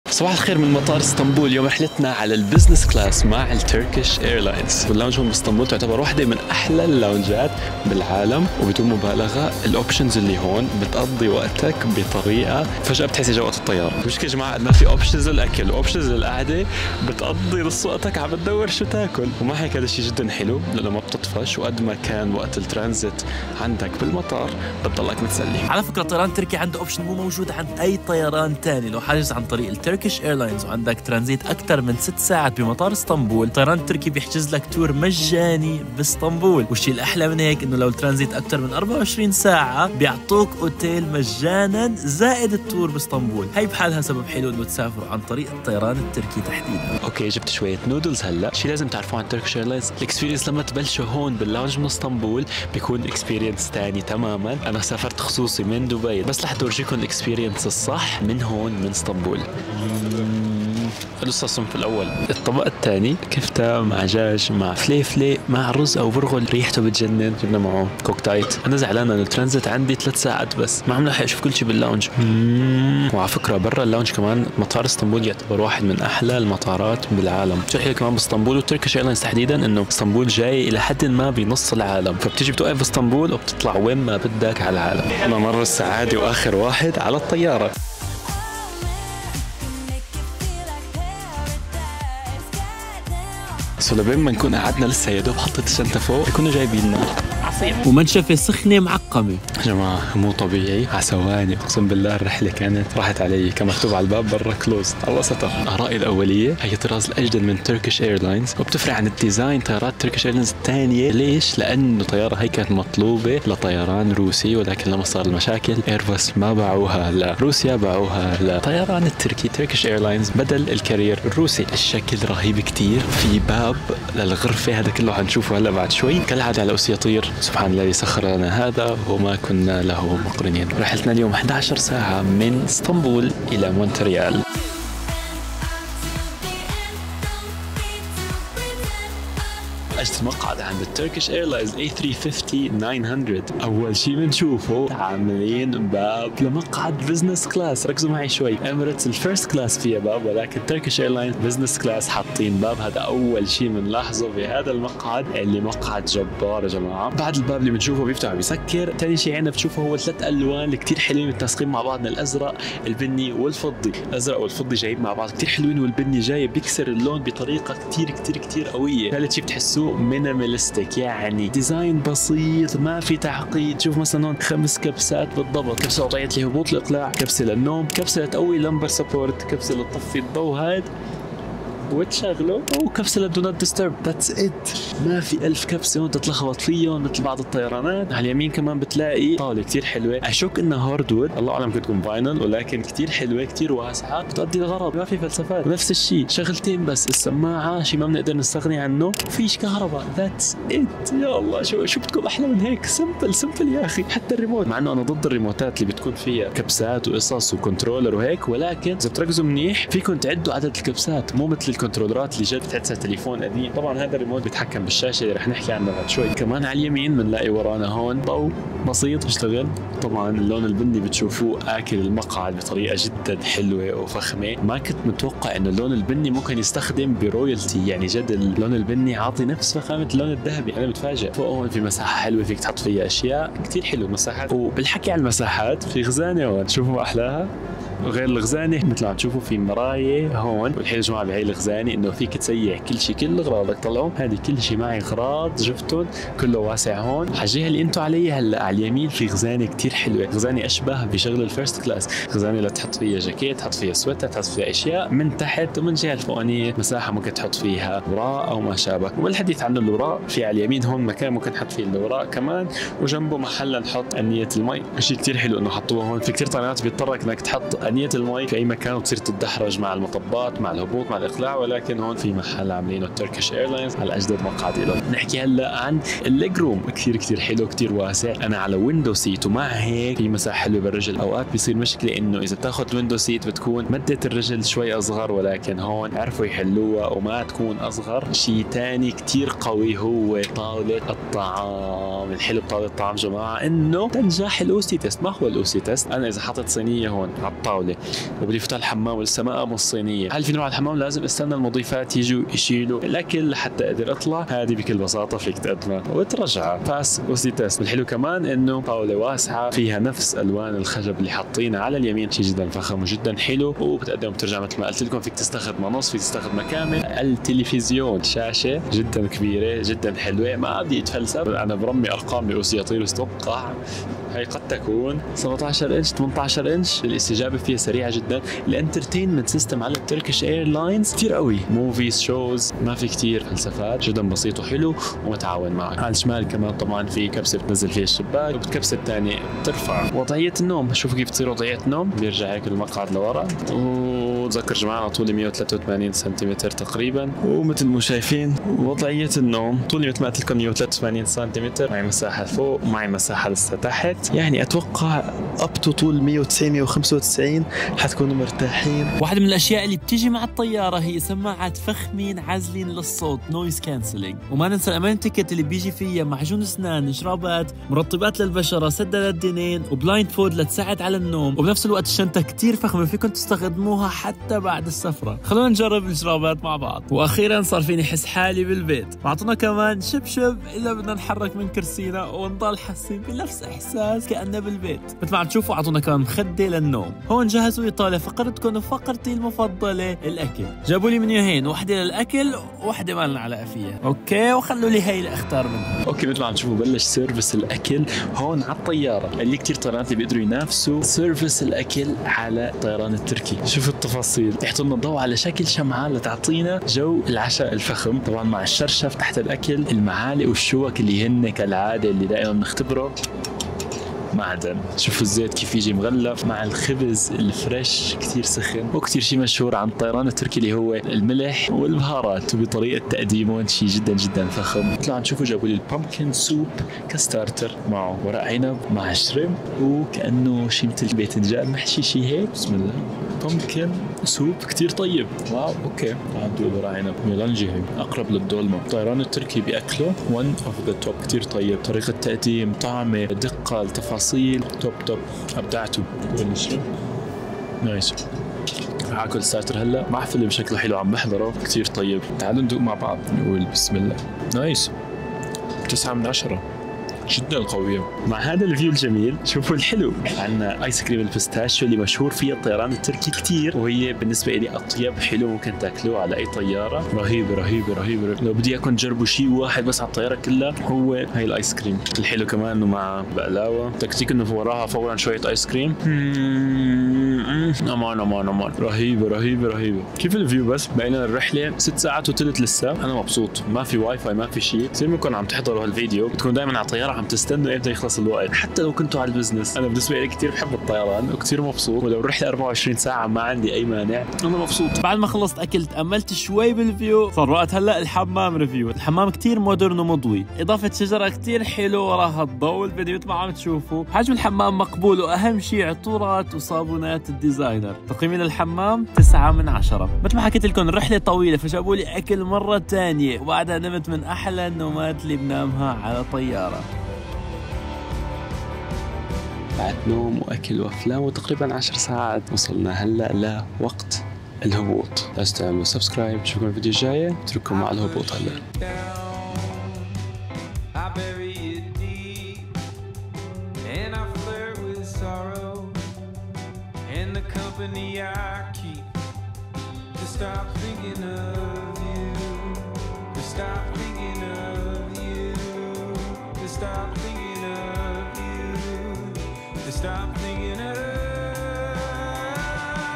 🇹🇷 تجربتي على درجة رجال الاعمال في الطيران التركي (3000$)
Thank you to Turkish Airlines for this opportunity!

سعر التذكره ممكن يتغير حسب الوجهه و التاريخ *
الاسعار محوله من الدرهم الاماراتي الى الدولار الامريكي *